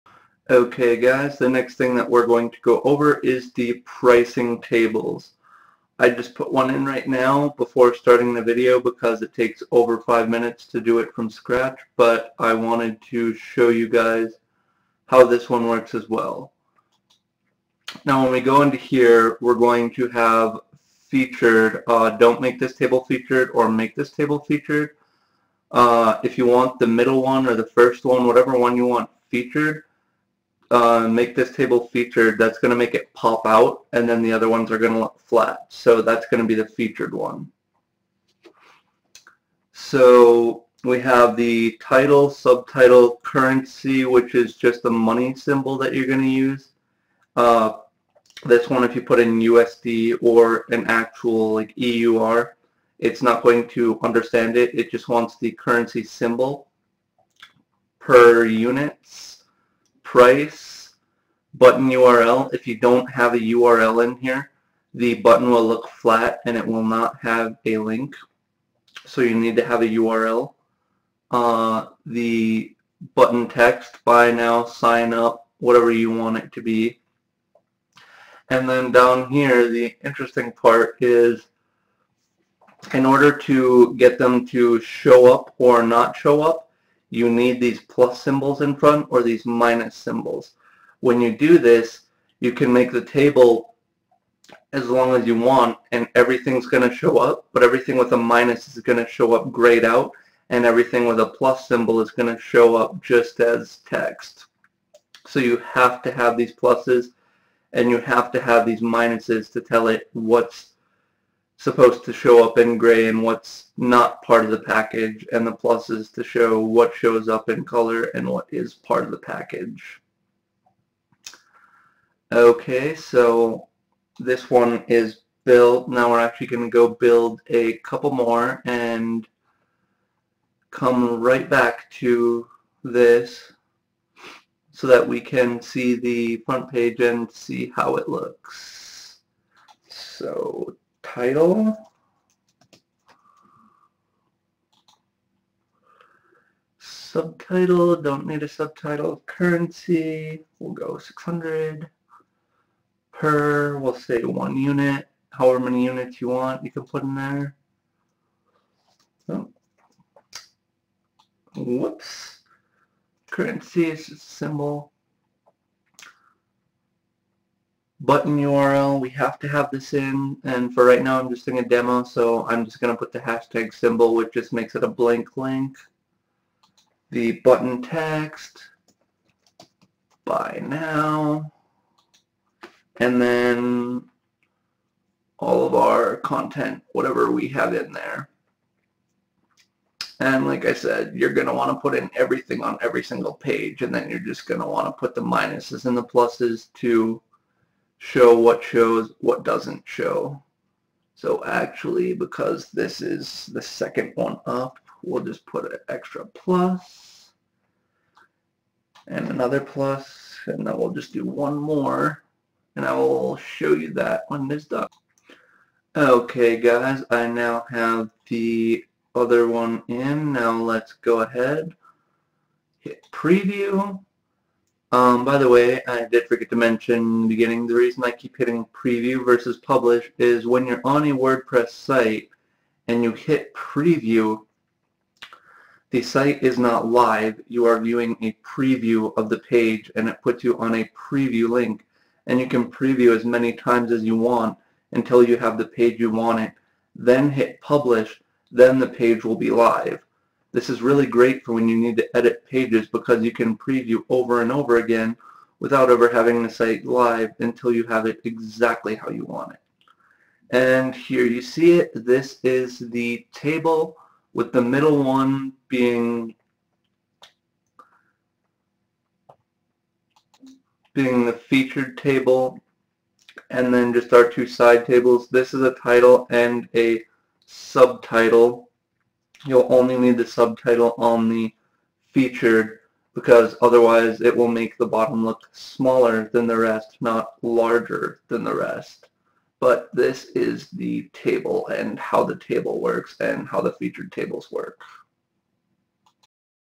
Divi How to Set Up Tables
This video goes over how to set up tables using the Divi Page Builder and how to set your featured table. I also go over how to tell the system what should not be grayed out and what should be grayed out in your tables.